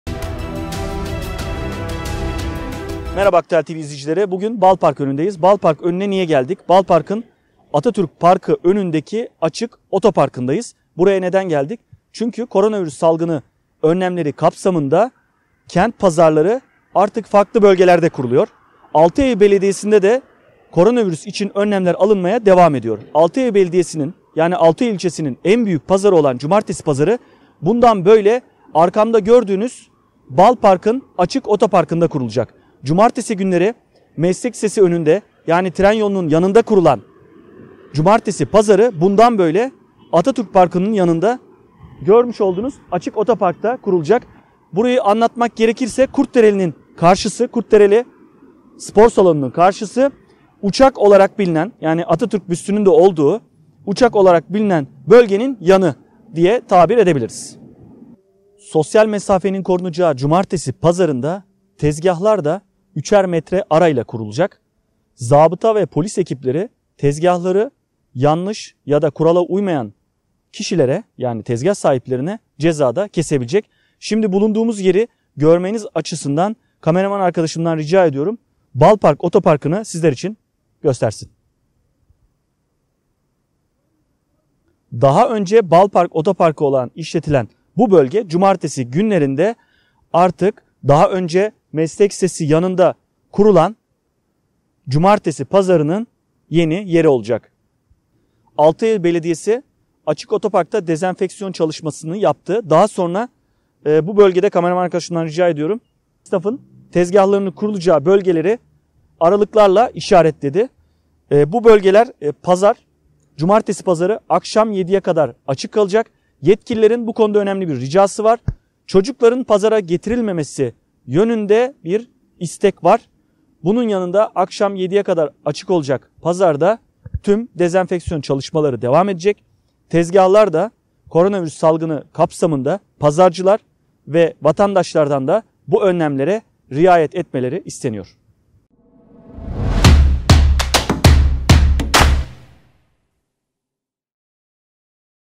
CUMARTESİ PAZARI BALPARK OTOPARKINDA KURULACAK
Koronavirüs salgını sonrasında Pazarlar içinde önlemler alındı. Bu kapsamda Altıeylül ilçesinin en büyük pazarı olan Cumartesi Pazarı bu artık farklı bir yerde kurulacak.

Daha önce tren yolu yan tarafında yani meslek lisesi önünde kurulan Cumartesi pazarı ikinci bir karara kadar Atatürk Parkı yanında ki Balpark Otoparkında kurulacak. Bazı vatandaşlarımızın bilmesi açısından Uçağın yanında kurulacak.

Sosyal mesafenin korunacağı Cumartesi pazarında ise pazarcı sayısının iki katı kadar vatandaşa alışveriş için izin verilecek.  Yetkililer  çocuklar ile pazara gelinmemesini istiyor. Pazar akşam saat 7’ye kadar açık kalacak. Ve zabıta ve Polis ekipleri Koronavirüs önlemlerine karşı tezgahların 3 metre mesafe ile kurulması gerekiyor. Ayrıca ürünler göz ile seçilecek.
Haber: Süleyman Namlı
Kamera: Şeyda Aslan